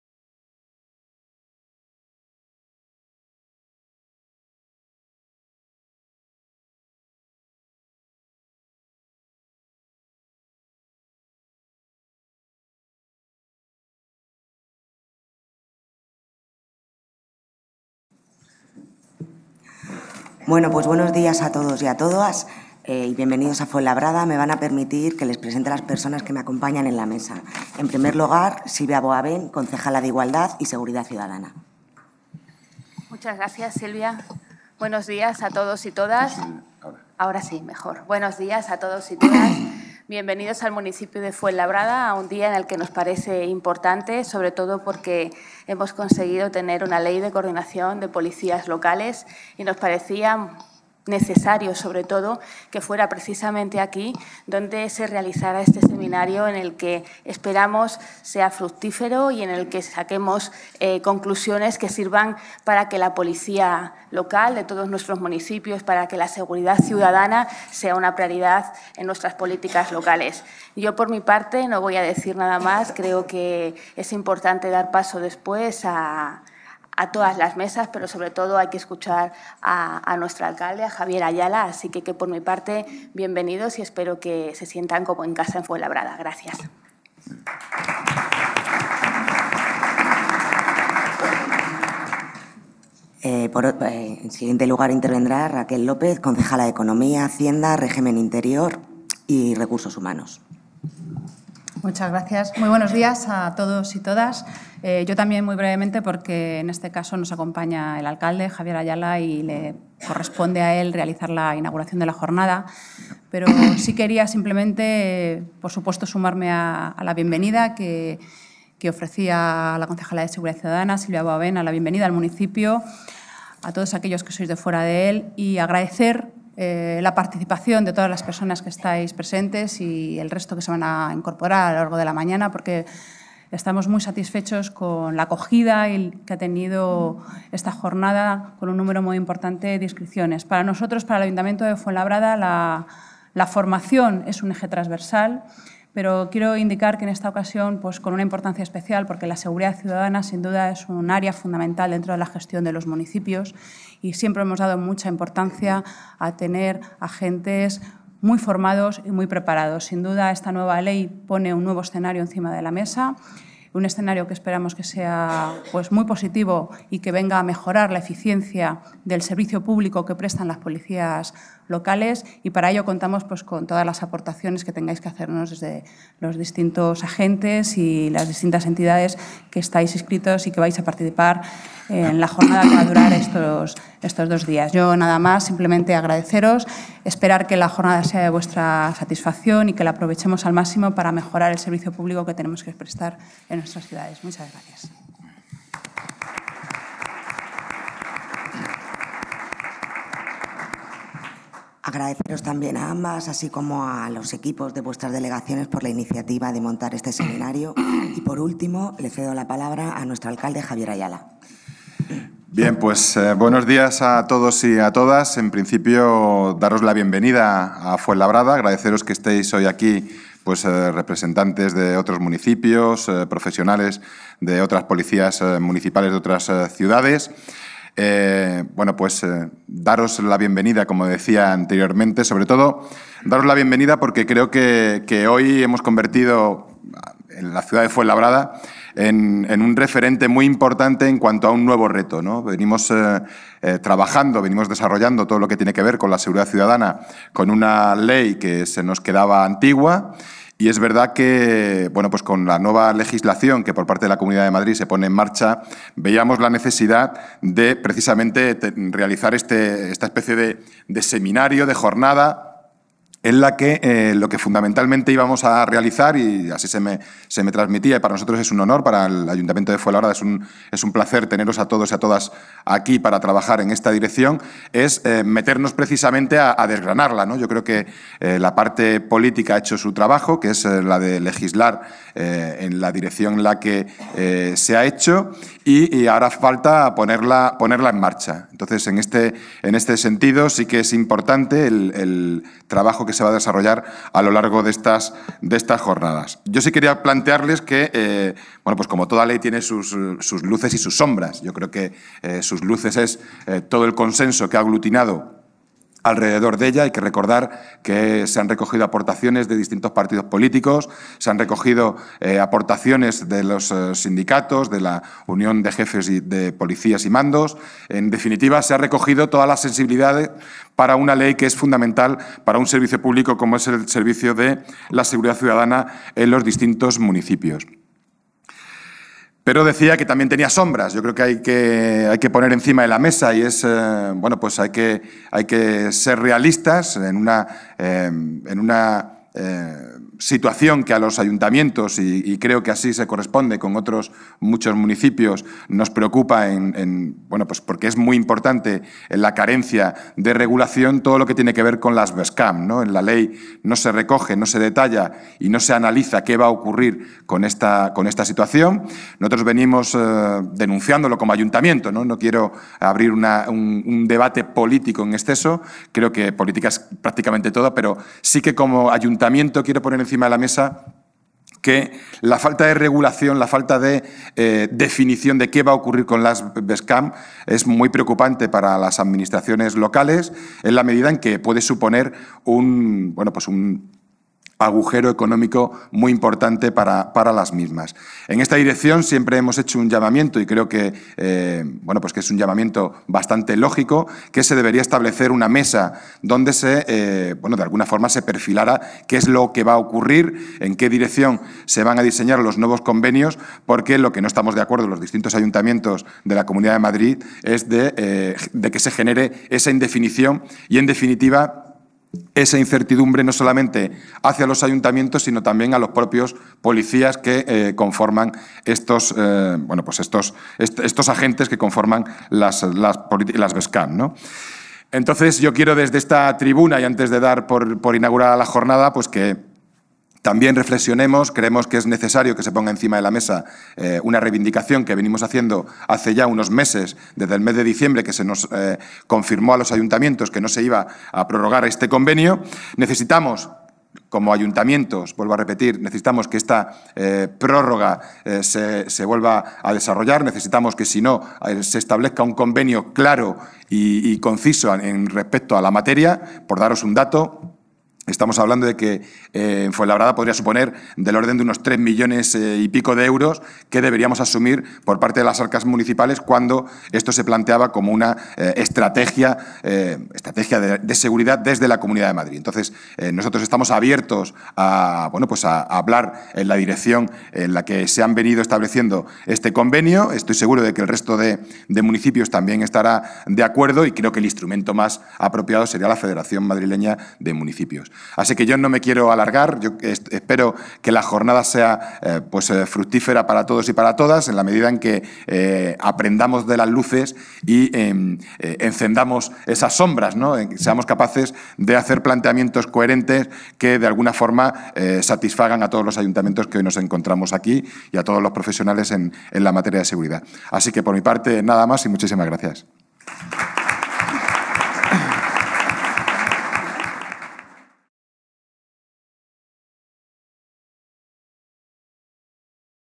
Acto inaugural - Seminario Ley Coordinación Policías Locales de la Comunidad
Acto inaugural del Seminario de la nueva Ley de Coordinación de Policías Locales de la Comunidad de Madrid.